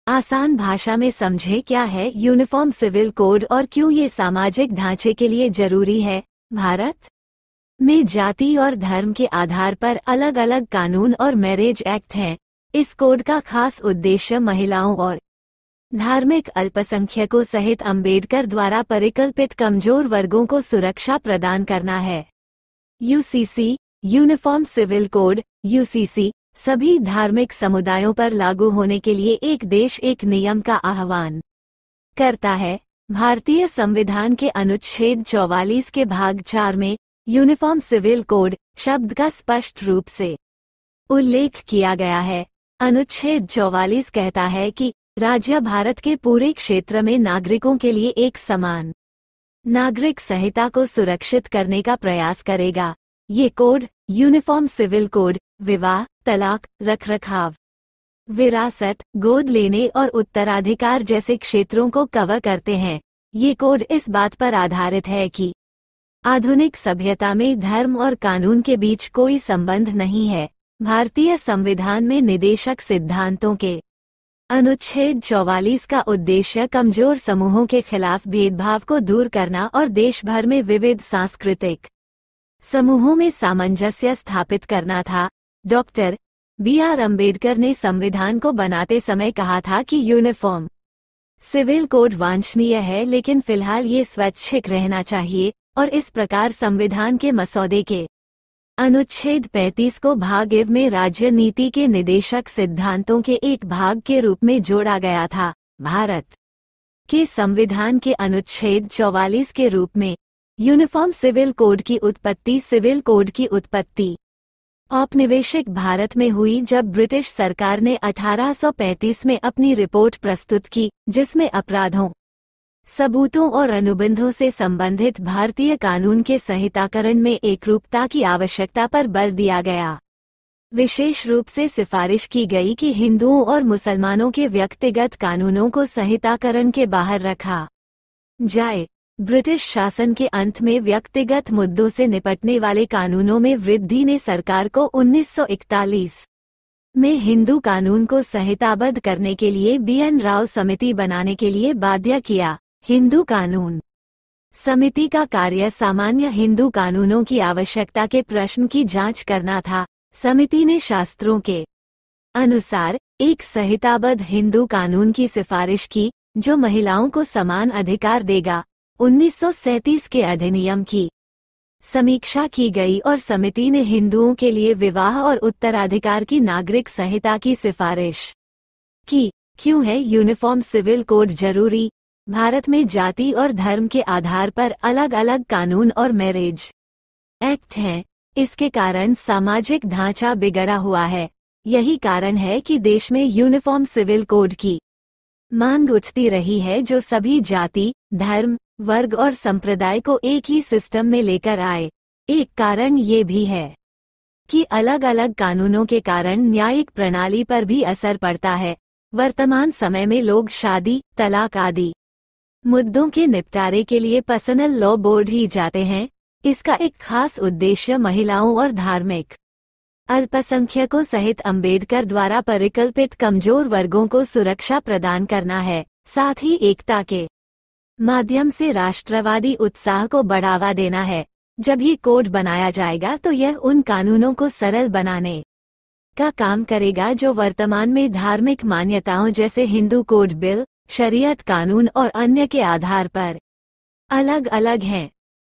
क्यों है यूनिफॉर्म सिविल कोड जरूरी?@SarojYadav1176
भारत में जाति और धर्म के आधार पर अलग-अलग कानून और मैरिज एक्ट हैं। इसके कारण सामाजिक ढ़ांचा बिगड़ा हुआ है। यही कारण है कि देश में यूनिफॉर्म सिविल कोड की मांग उठती रही है जो सभी जाति, धर्म, वर्ग और संप्रदाय को एक ही सिस्टम में लेकर आए। एक कारण यह भी है कि अलग-अलग कानूनों के कारण न्यायिक प्रणाली पर भी असर पड़ता है। वर्तमान समय में लोग शादी, तलाक आदि मुद्दों के निपटारे के लिए पर्सनल लॉ बोर्ड ही जाते हैं। इसका एक खास उद्देश्य महिलाओं और धार्मिक अल्पसंख्यकों सहित अम्बेडकर द्वारा परिकल्पित कमजोर वर्गों को सुरक्षा प्रदान करना है, साथ ही एकता के माध्यम से राष्ट्रवादी उत्साह को बढ़ावा देना है। जब यह कोड बनाया जाएगा तो यह उन कानूनों को सरल बनाने का काम करेगा जो वर्तमान में धार्मिक मान्यताओं जैसे हिंदू कोड बिल, शरीयत कानून अन्य के आधार पर अलग-अलग हैं। और अन्य के आधार पर अलग-अलग हैं।